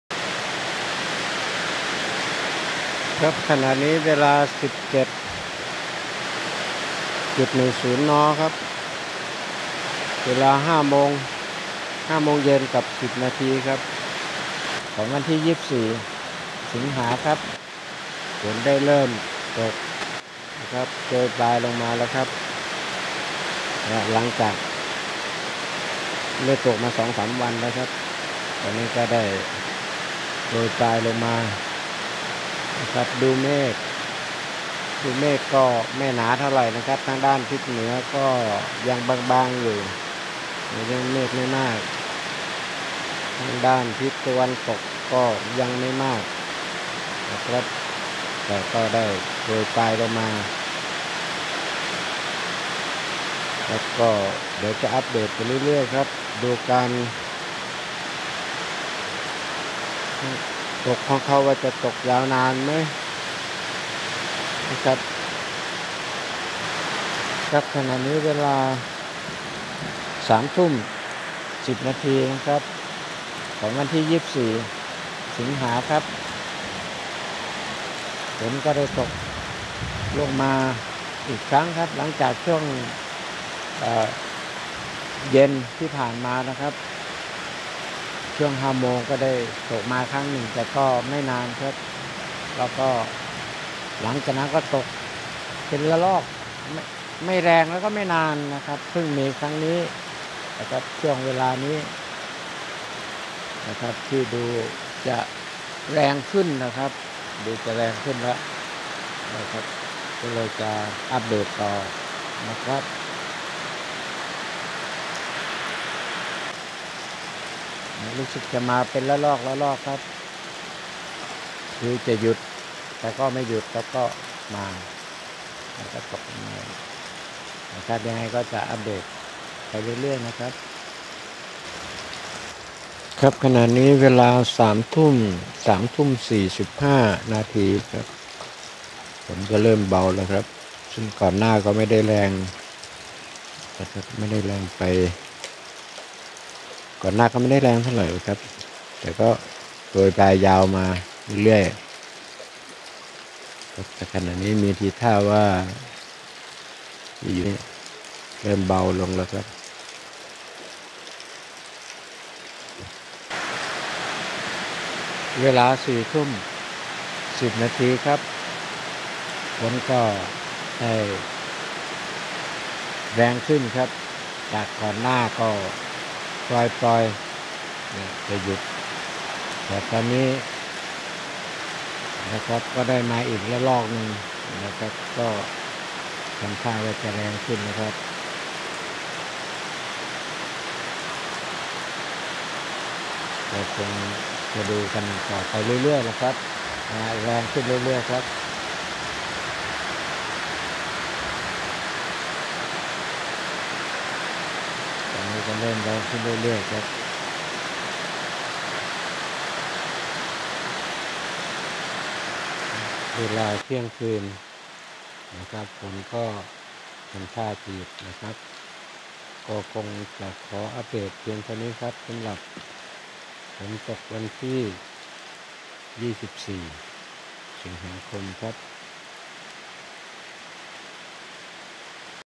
ฝนตก 24 ส.ค.65 ที่บ้านถาวร
ฝนตกที่บ้านถาวร ตำบลถาวร อำเภอเฉลิมพระเกียรติ จังหวัดบุรีรัมย์